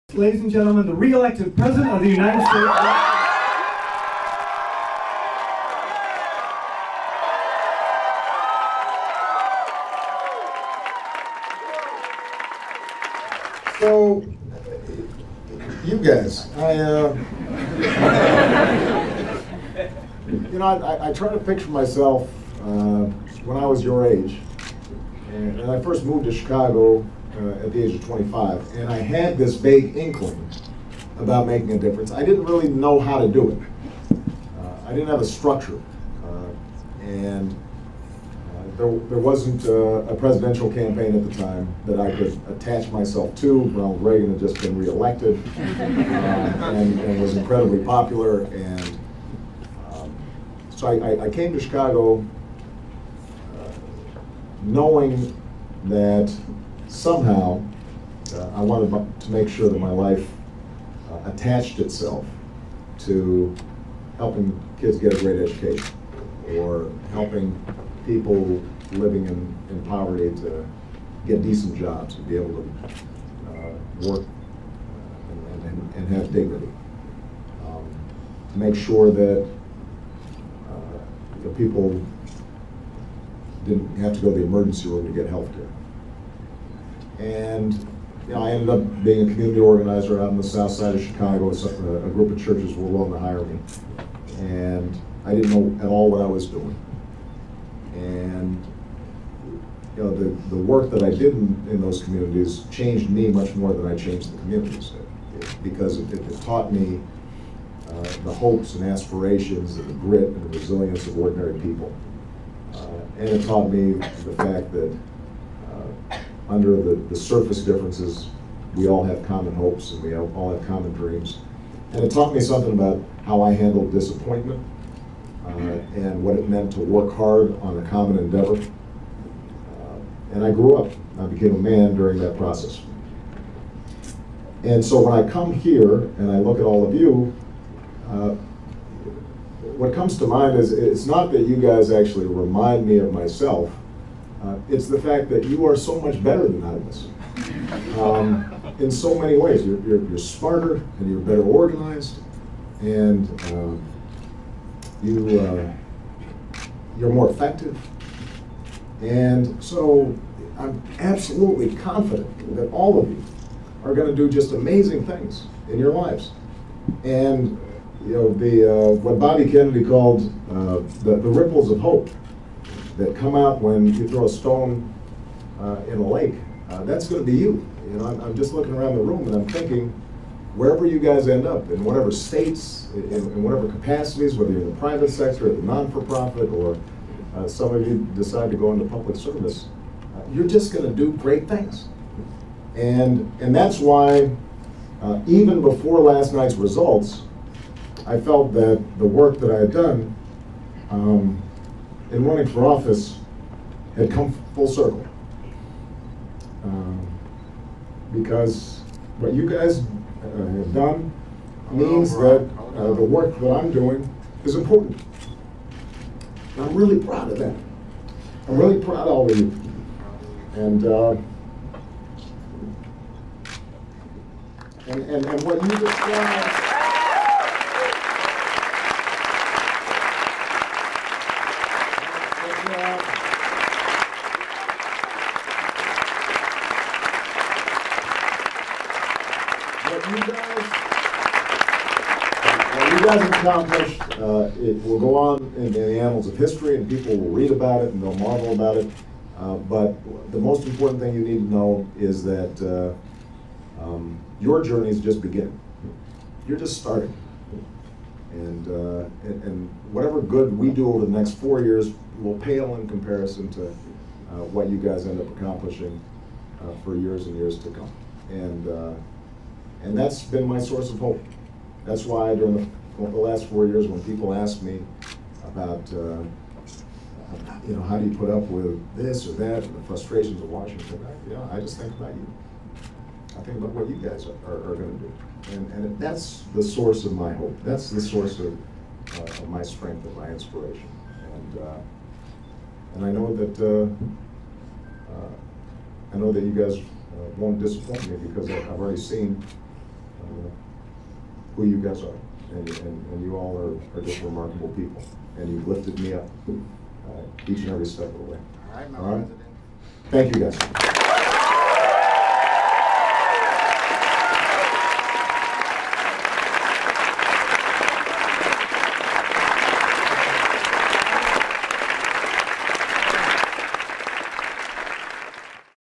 Obama's Tearful "Thank You" To Campaign Staff
President Obama thanks his campaign staff at the campaign's headquarters in Chicago, Ill.